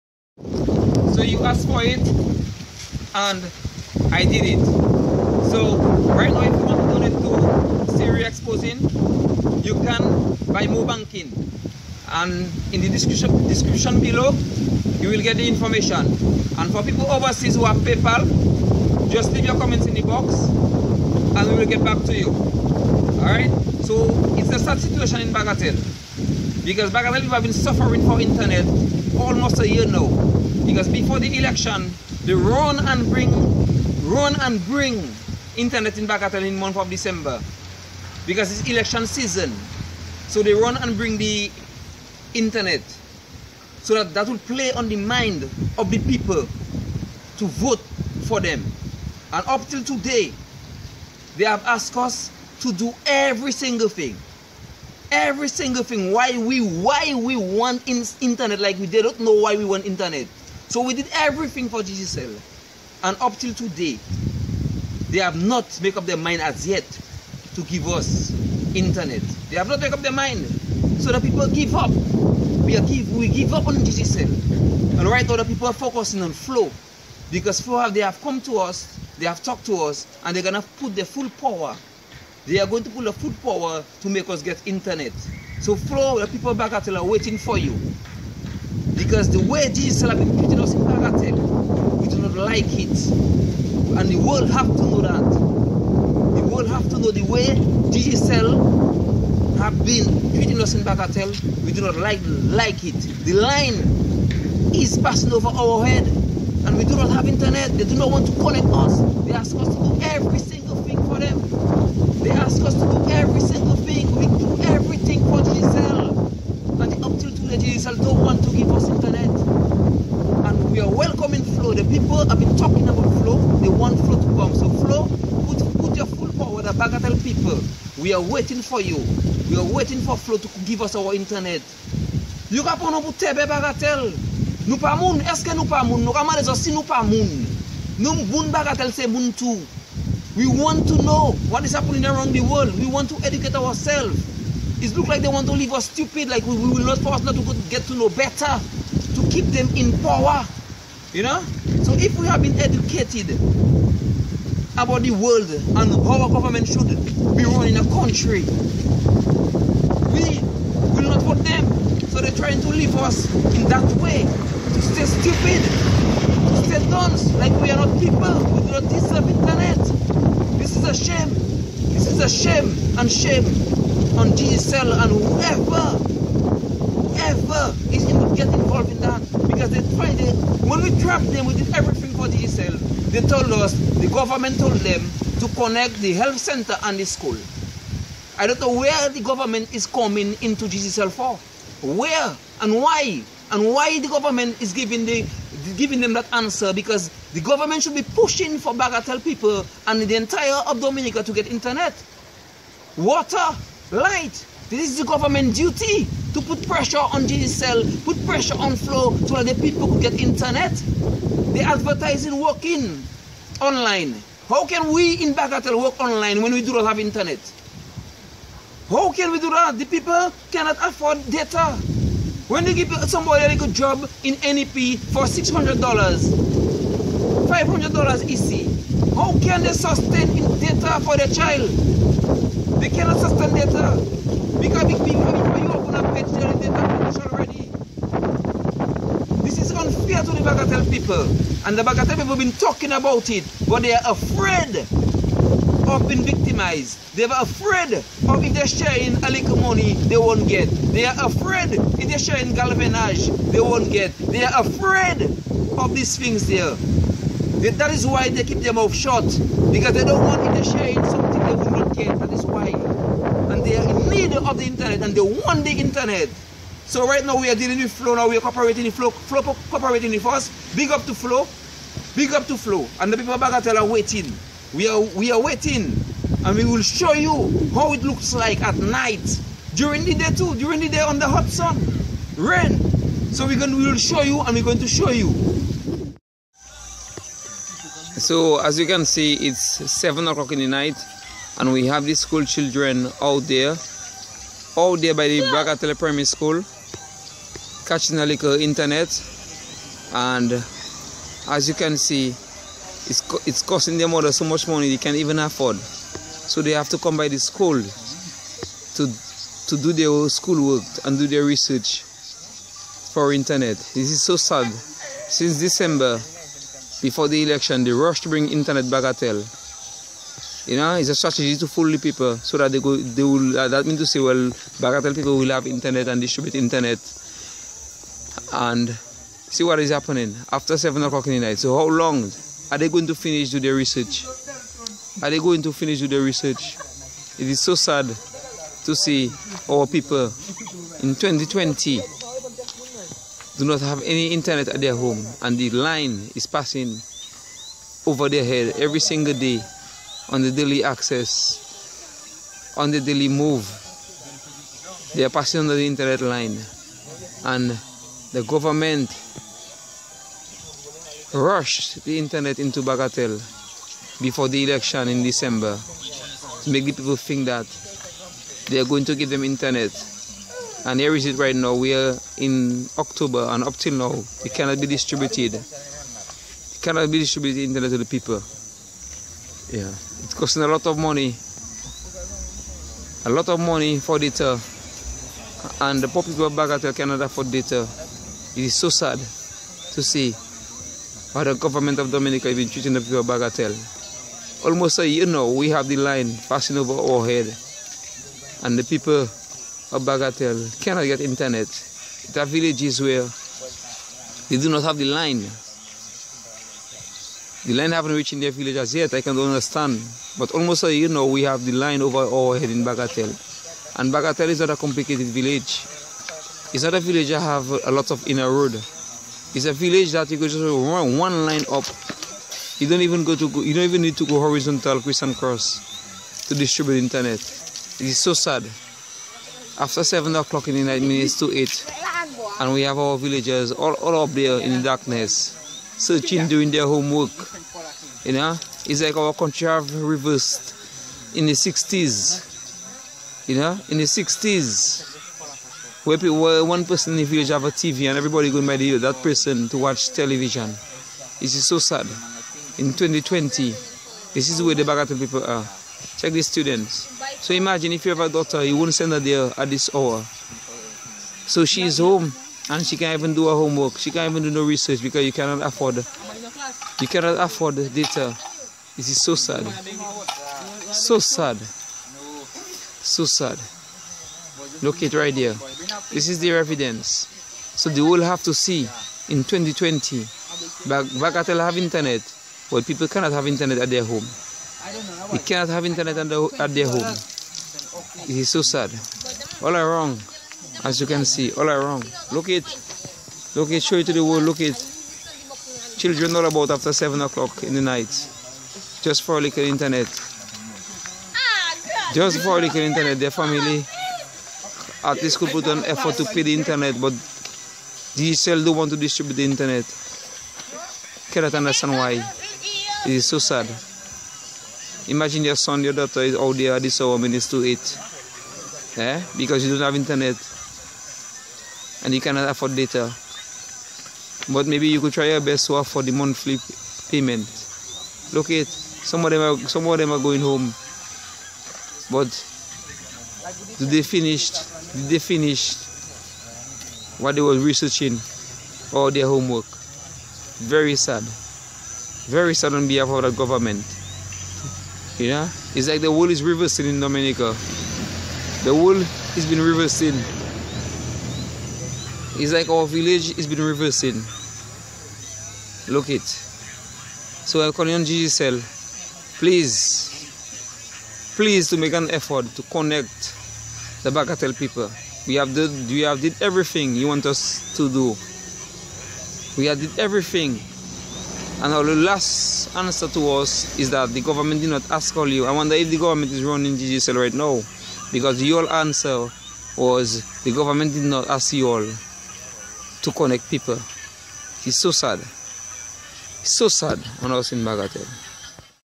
Frustration: Digicel Playing Games with Bagatelle People: Still NO INTERNET!
Siri Exposing Digicel and the lack of professionalism for the need for internet. Bagatelle needs internet and has not had internet since Hurricane Maria.